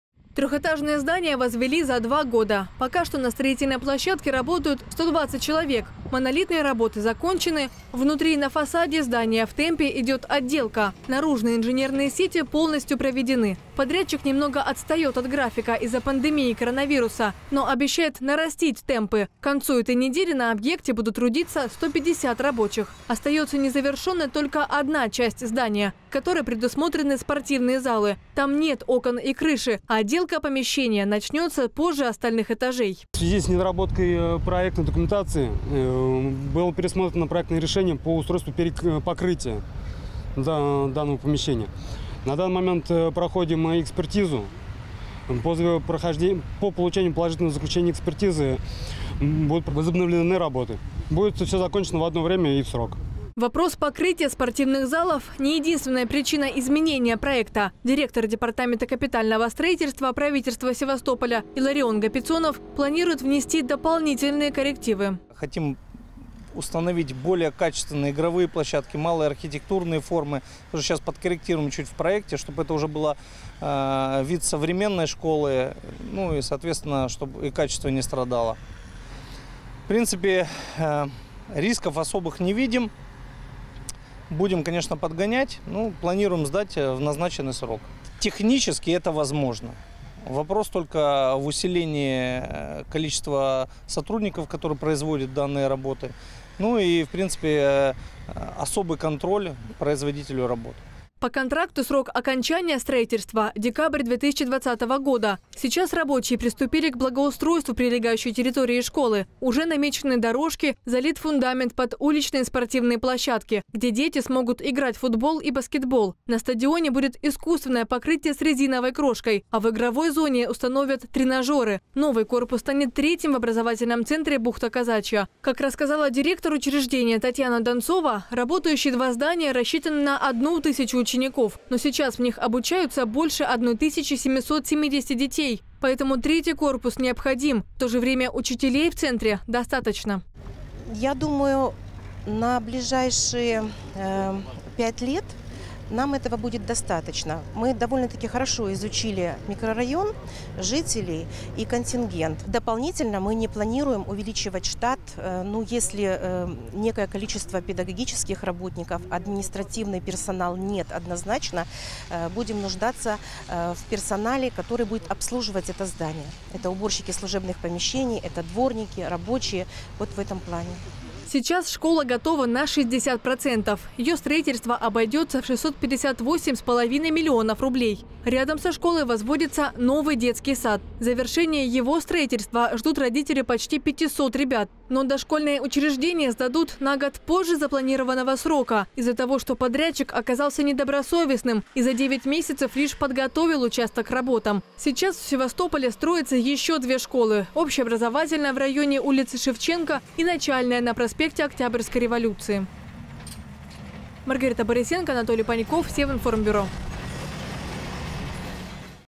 Подрядчик строительства школы в районе бухты Казачьей отстаёт от графика
Дополнительный корпус школы в районе бухты Казачьей готов на 60%. Несмотря на небольшое отставание от графика, подрядчик обещает закончить строительство до конца этого года. Образовательное учреждение рассчитано на 704 ученика. В школе оборудуют мастерские по обработке металла и древесины, кабинеты домоводства, музыкальный и зрительный залы, комнату психологической разгрузки, фотолабораторию, тир, игровую и физкультурную площадки. Сейчас строители занимаются отделкой помещений и фасада.